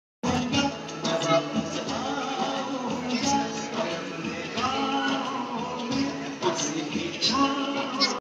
My Cart wheel performance in a dry swimming pool.
When you decide that you want to do Cart wheel you need lot of practice and of course falls getting hurt and what not however when you practice it long enough then you get perfection and all around will love it too..

Love Nevaeh